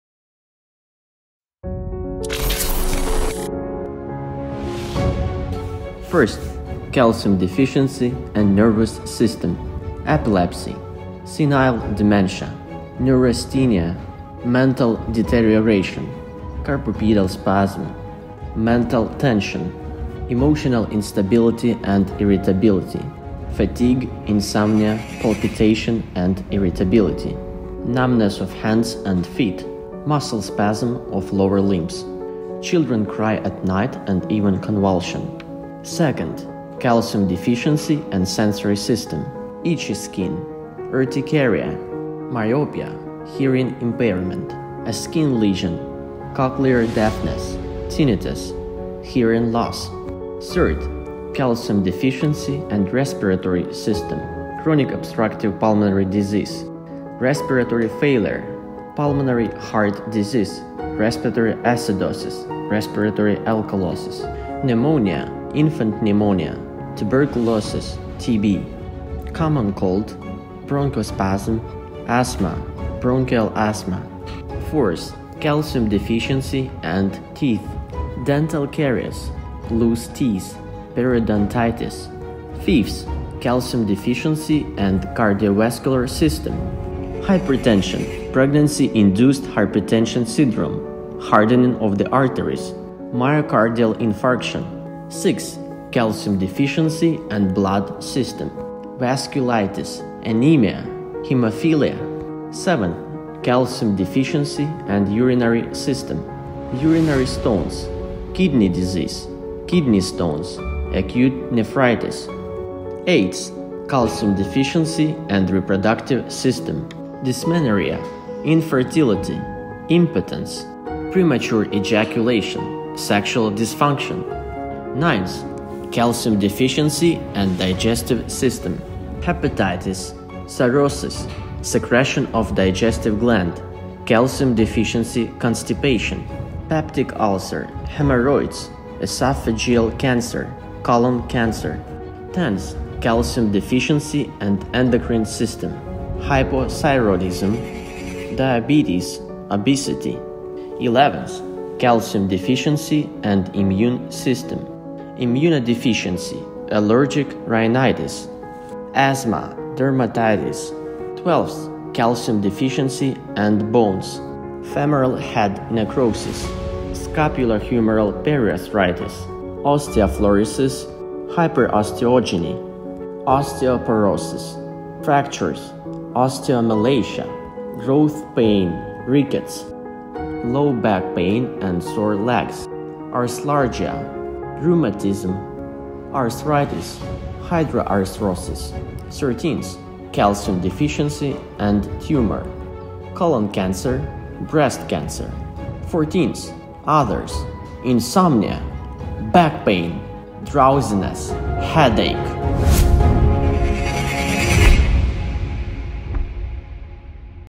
Diseases Caused By Calcium Deficiency .... 英文版
" Research Shows More Than 100 Diseases Caused by Calcium Deficiency , Some of Them Shows Very Well in The Amazing Video "